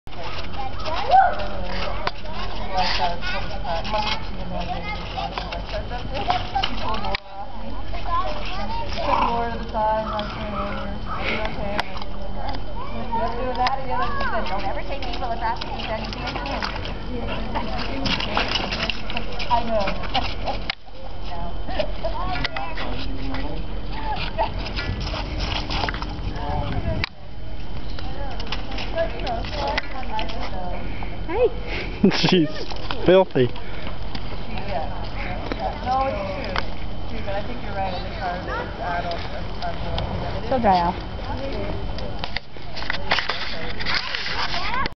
Muddy Finlay
Finlay covered in mud at Point Plesant Park on March 28th 2009